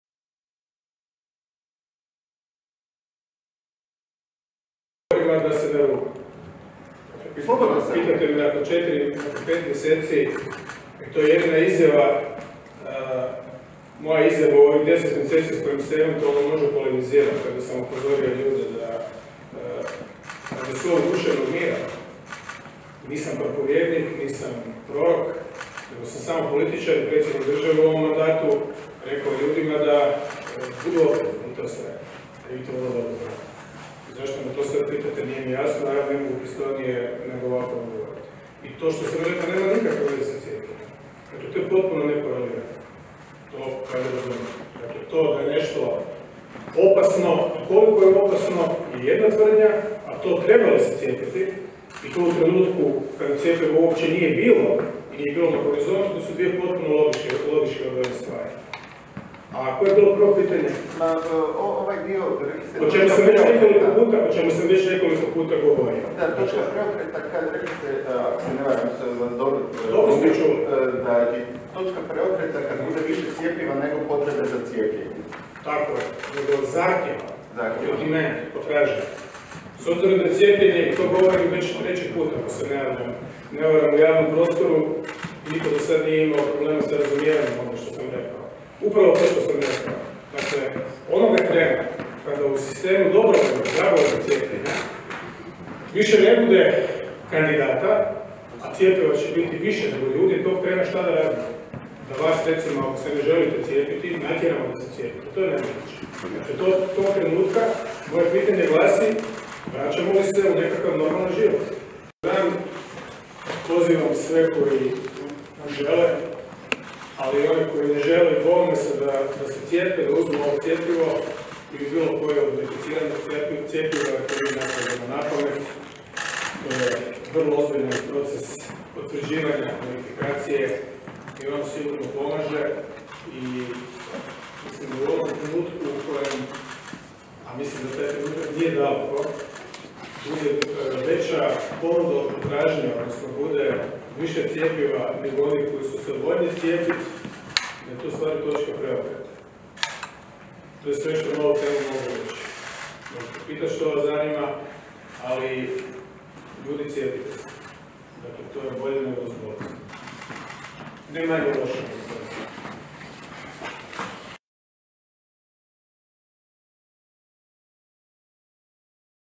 Predsjednik Milanović pozvao je građane da se cijepe protiv koronavirusa
Predsjednik Zoran Milanović danas se u Uredu predsjednika javno cijepio protiv koronavirusa. Uz njega cijepio se i načelnik glavnog stožera Oružanih snaga Robert Hranj. Nakon cijepljenja predsjednik je pozvao građane da se cijepe protiv koronavirusa.